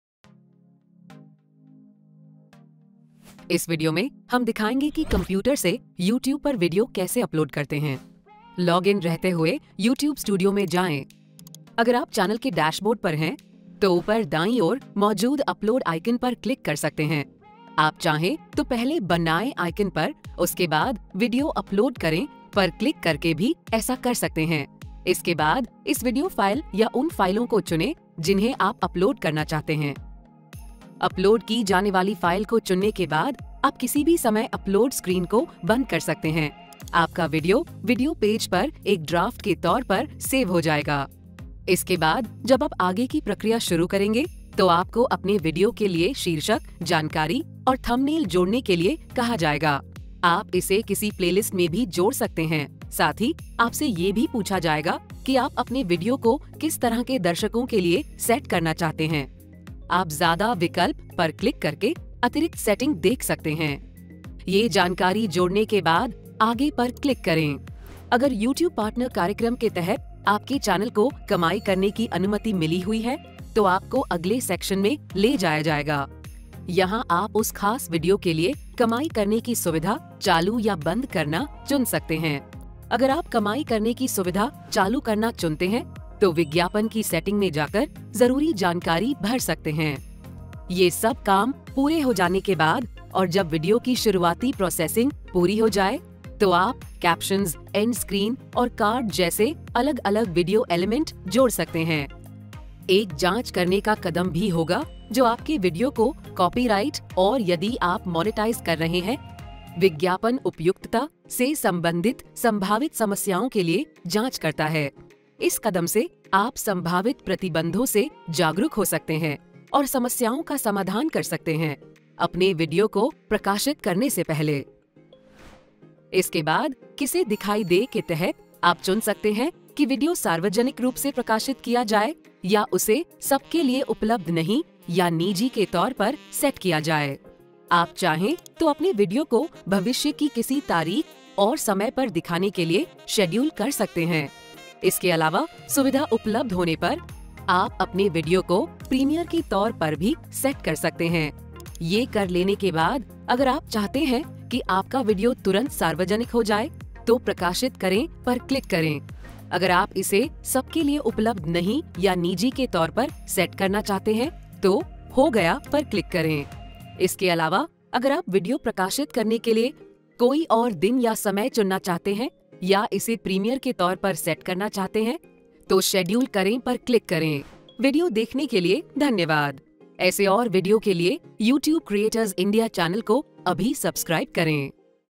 Creator Basics: How To Upload Videos with YouTube Studio | कंप्यूटर से यूट्यूब पर वीडियो अपलोड करें
In this video, we'll walk you through how to upload videos from your computer with YouTube Studio.

इस वीडियो में, हम आपको बताएंगे कि YouTube स्टूडियो के साथ अपने कंप्यूटर से वीडियो कैसे अपलोड करें। हम आपको यह भी दिखाएंगे कि आप कैप्शन, कार्ड और एंड स्क्रीन कहां जोड़ सकते हैं, वीडियो को प्लेलिस्ट में कैसे जोड़ सकते हैं, उनकी विज्ञापन-उपयुक्तता या किसी भी कॉपीराइट मुद्दों की जांच कर सकते हैं, और बहुत कुछ।